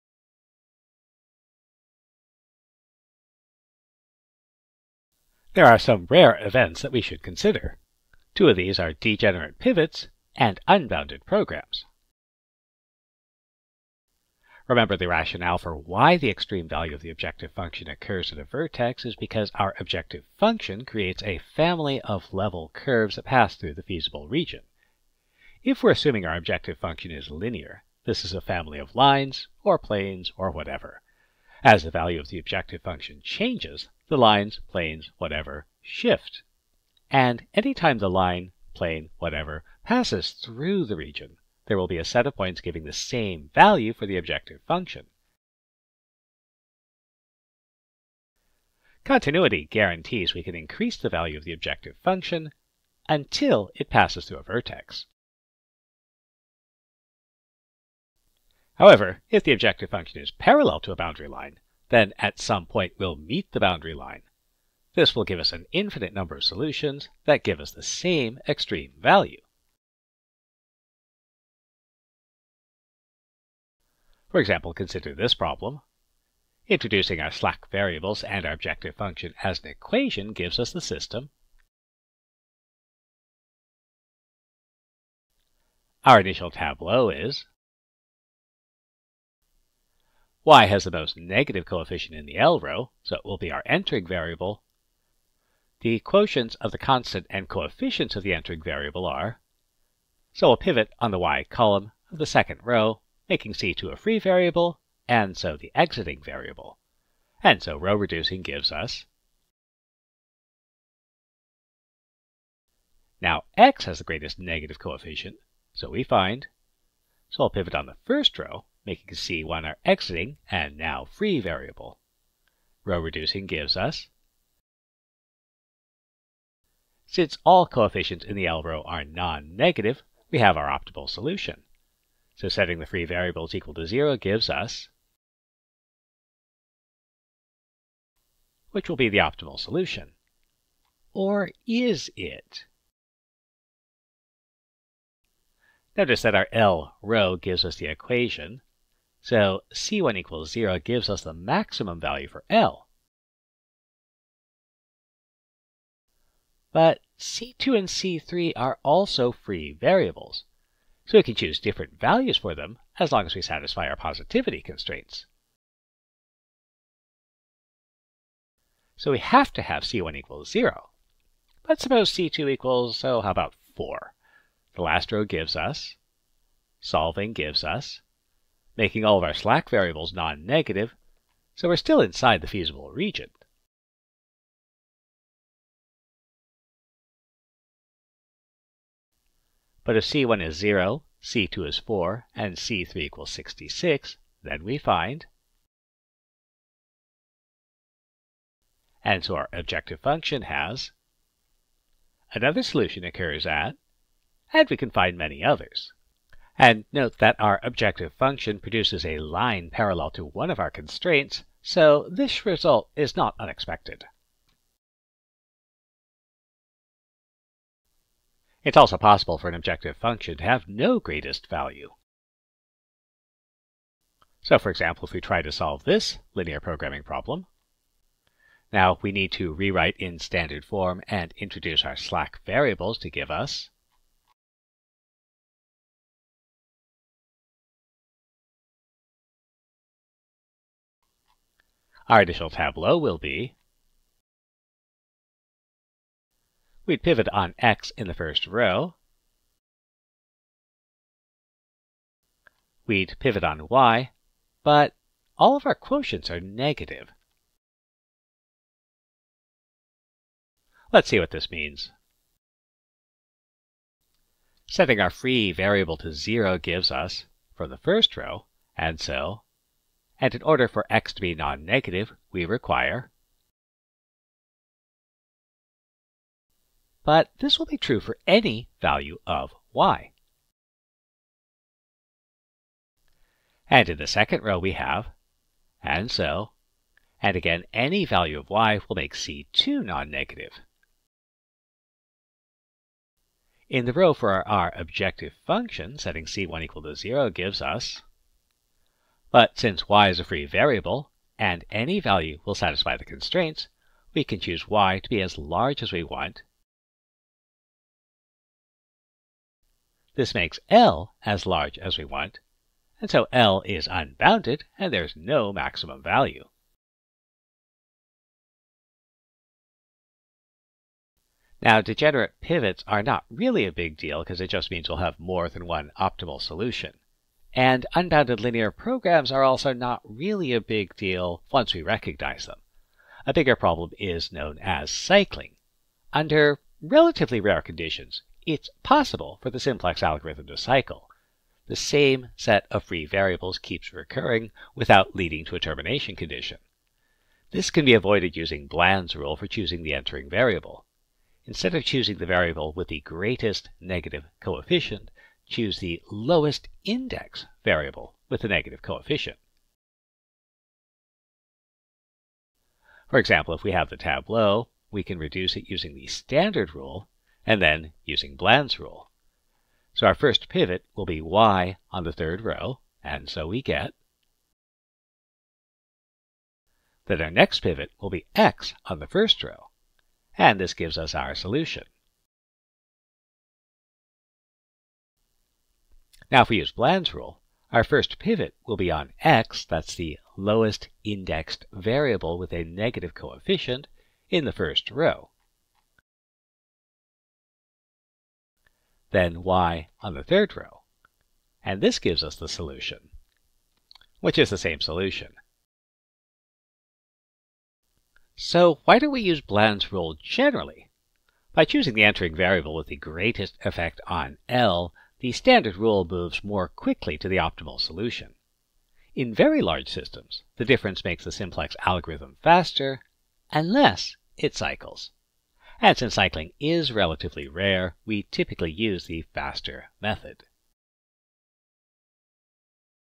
The Simplex Algorithm: Degenerate Pivots, Unbounded Programs, and Cycling
Three relatively rare situations that happen with linear programming, and how to deal with them.